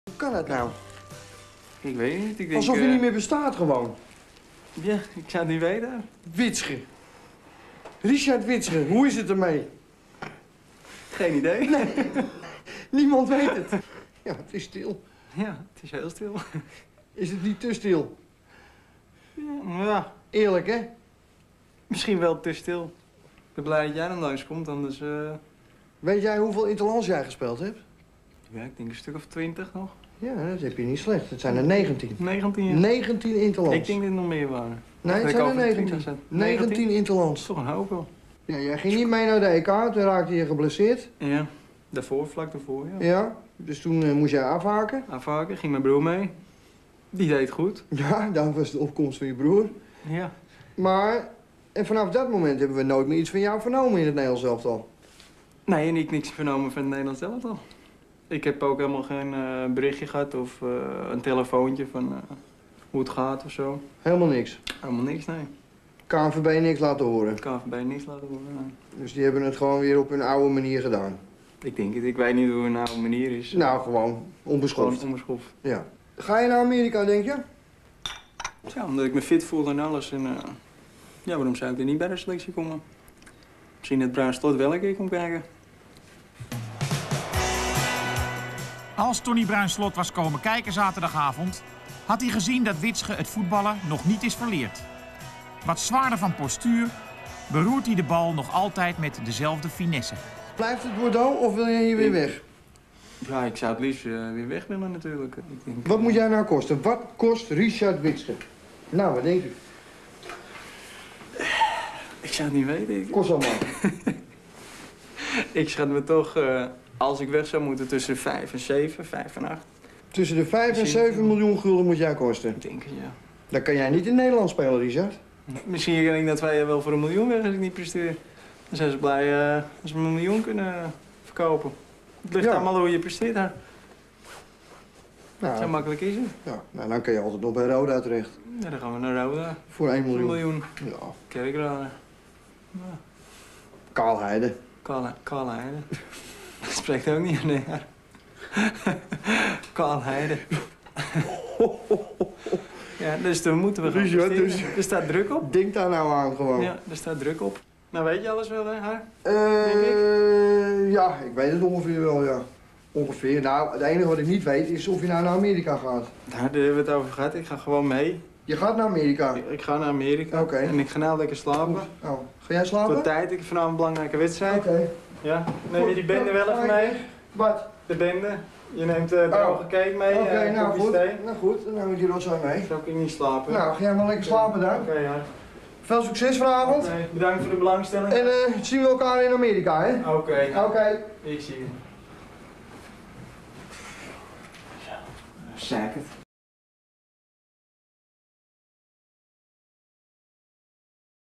Langs bij Richard Witschge in Bordeaux!
Bij Girondins de Bordeaux vind ik Richard Witschge. Het is al een tijdje stil rondom hem daarom zoek ik hem op!

Richard Peter Witschge (Amsterdam, 20 september 1969) is een voormalig Nederlands profvoetballer. Hij speelde vooral links op het middenveld.